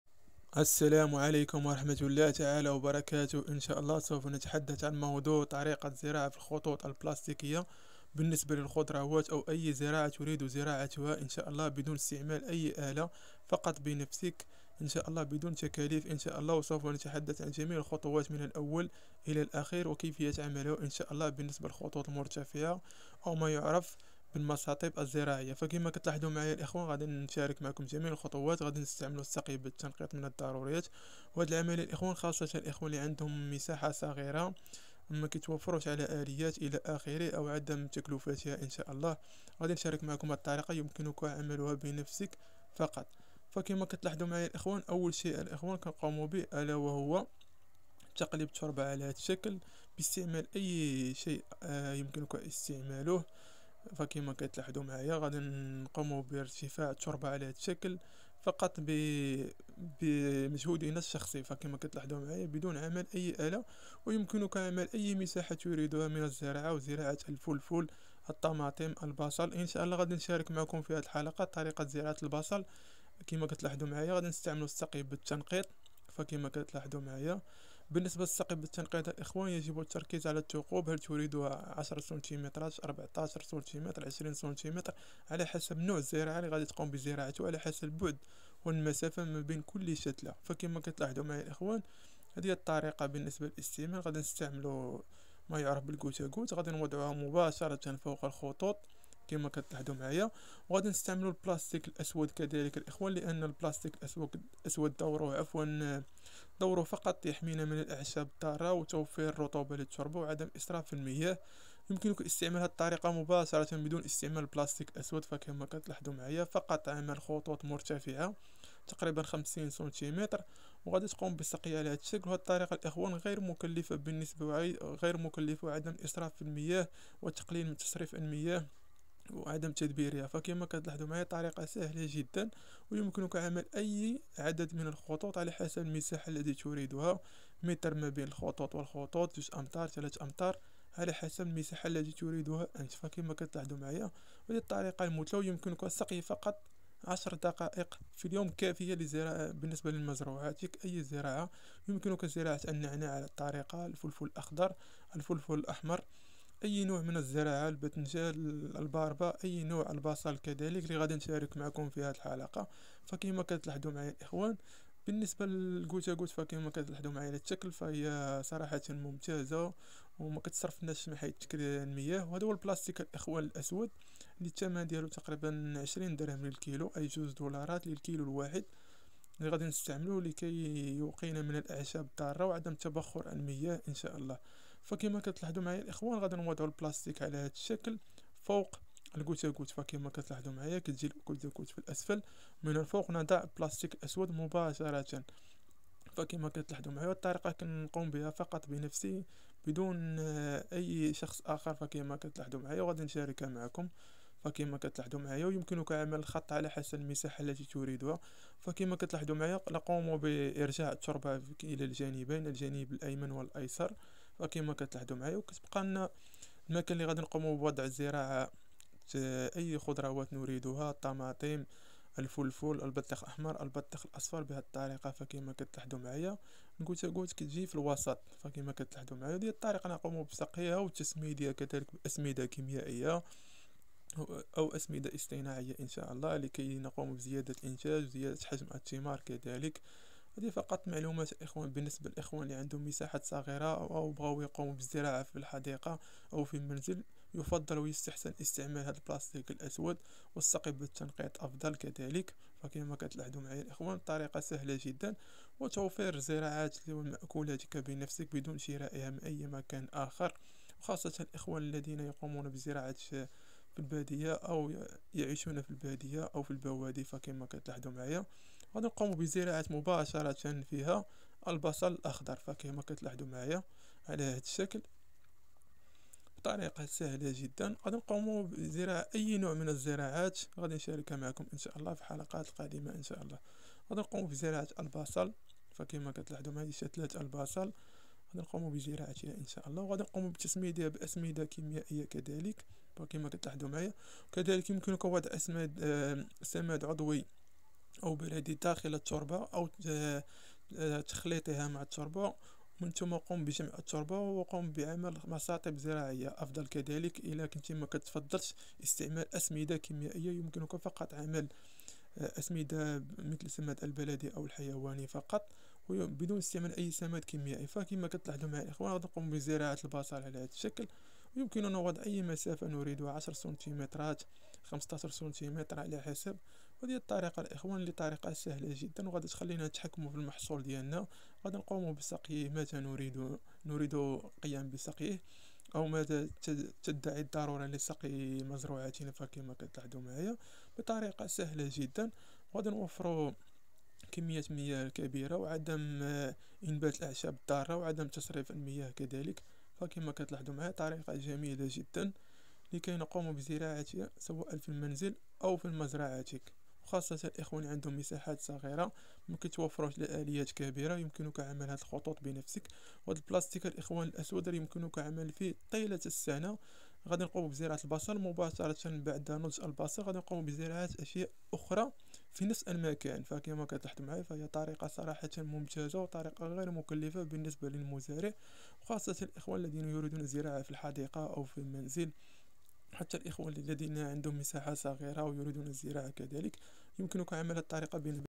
تجهيز الأرض الزراعية بخطوط بلاستيكية مرتفعة بدون جرار أو الألات بالمنزل بنفسك
سلام عليكم إخواني الكرام في هذه حلقة موضوع تجهيز الأرض الزراعية بخطوط بلاستيكية مرتفعة زراعة الخضروات سواء بالمنزل او المزرعة وخاصة لمن يمتلك قطعة أرض صغيرة ذلك بعمل بنفسك بدون إستعمال ألالات أو جرار .